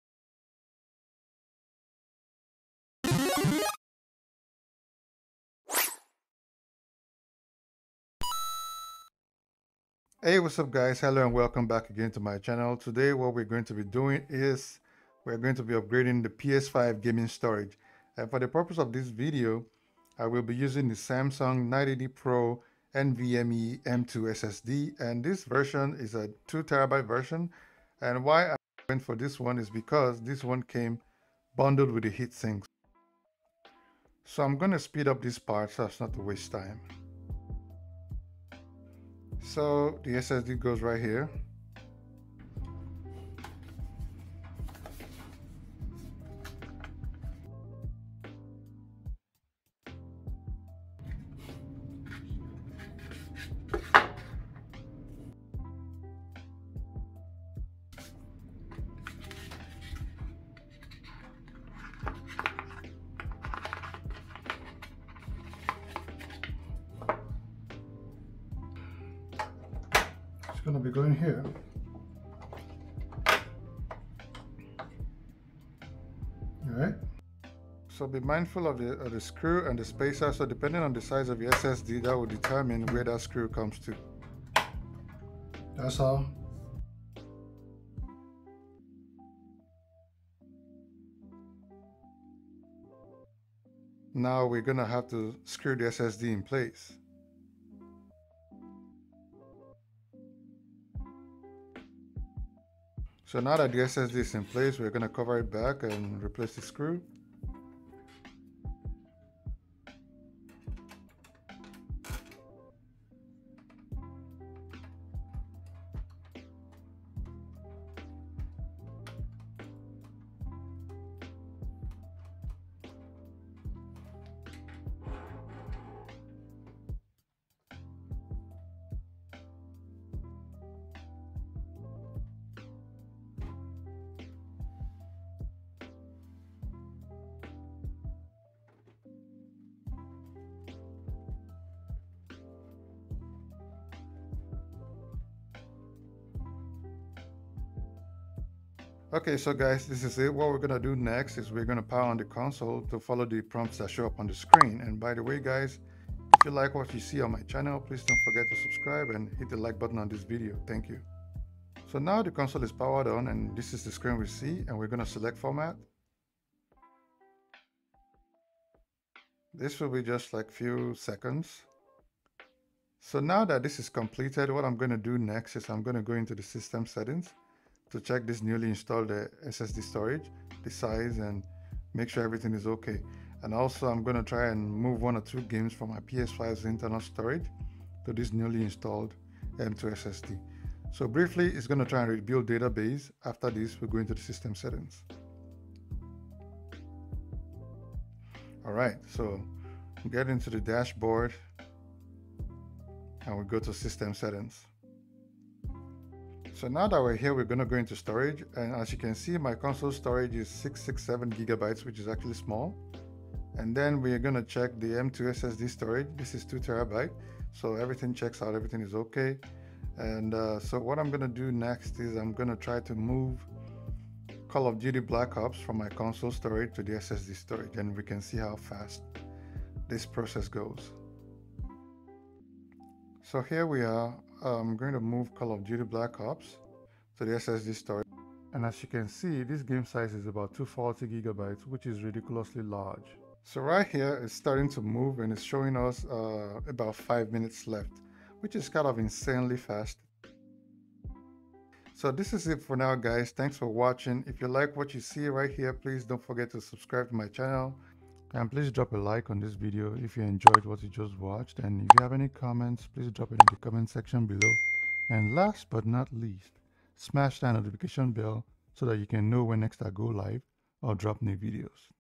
HOW TO UPGRADE THE PS5 CONSOLE STORAGE WITH M.2 SSD - COMPLETE GUIDE [4K]
Console storage upgrade guide using the Samsung Nvme M2 SSD 2TB.

- All my gaming gear and capture card used in one place